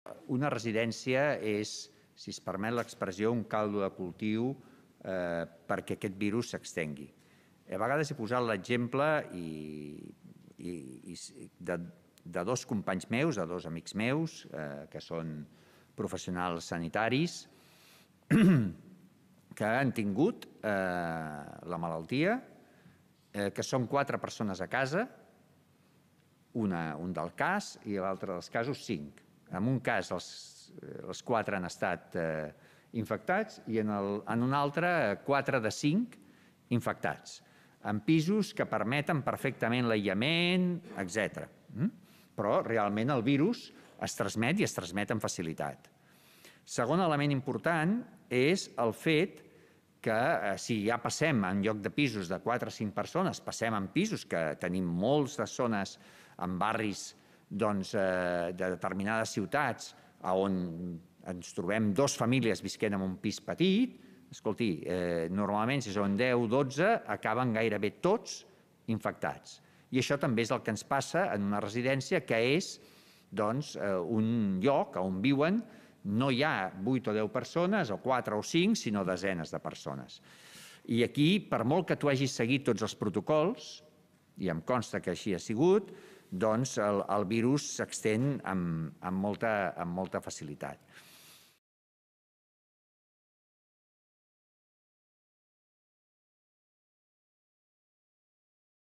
Perquè el coronavirus es propaga ràpidament en les residències de gent gran
Perquè el coronavirus es propaga ràpidament en les residències de gent gran. Vídeo: Dep. Salut